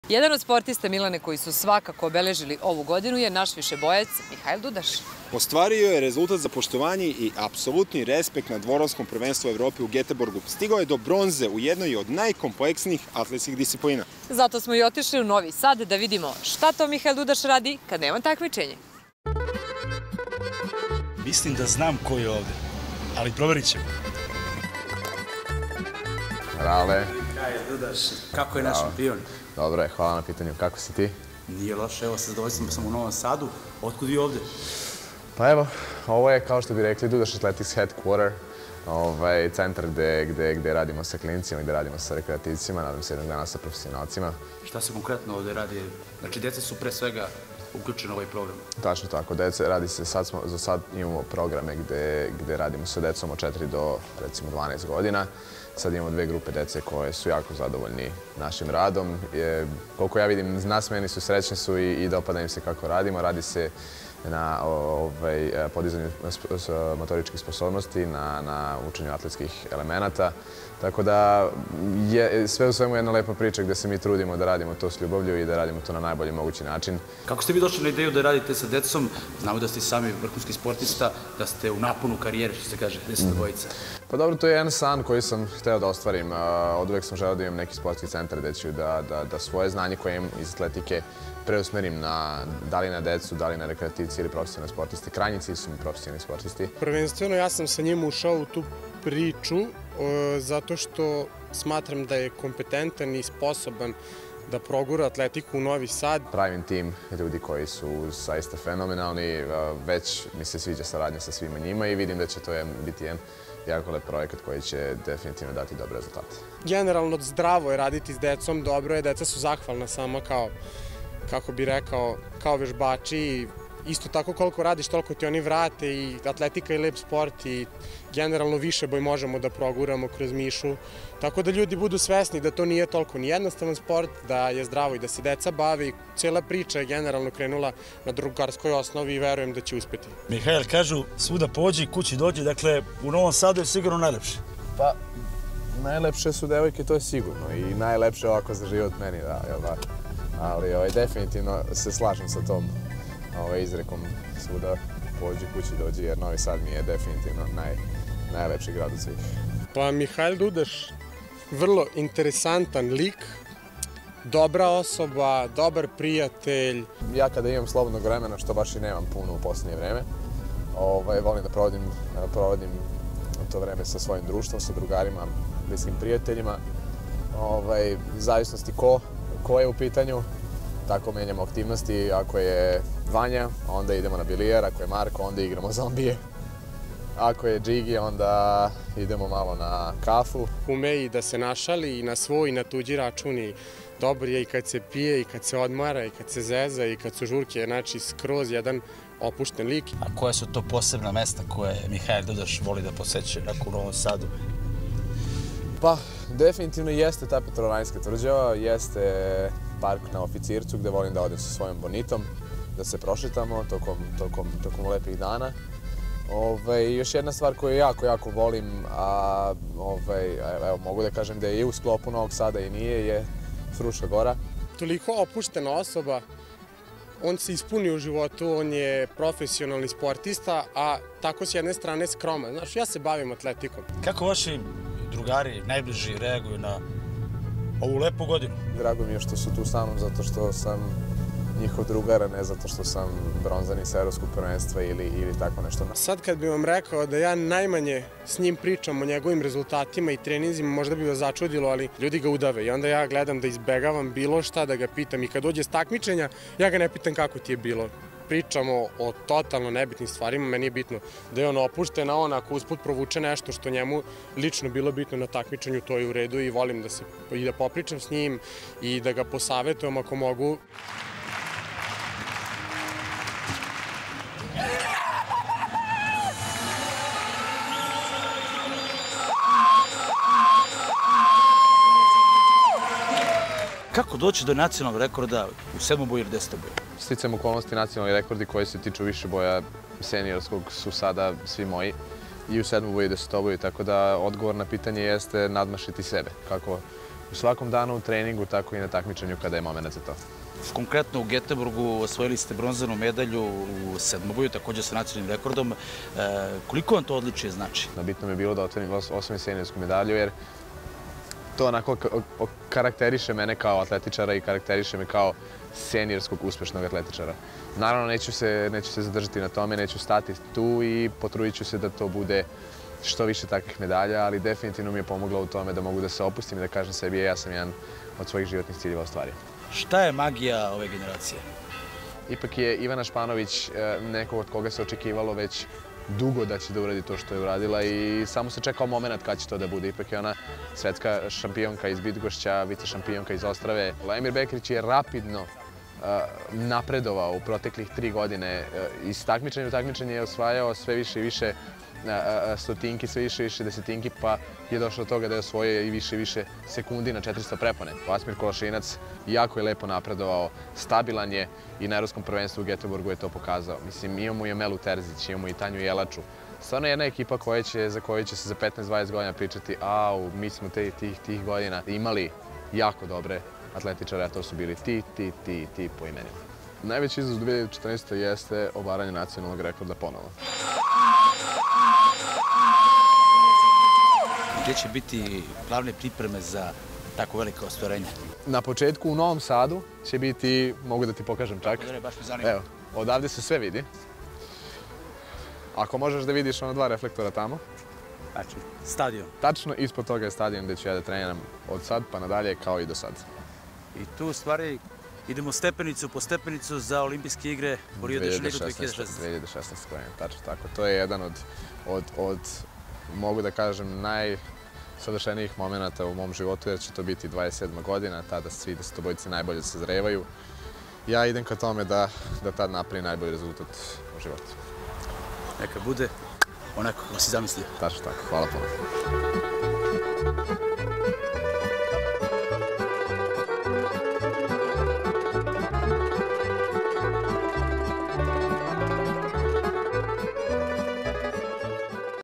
Mihail Dudaš
Pogledajte reportažu i intervju sa desetobojcem Mihailom Dudašem.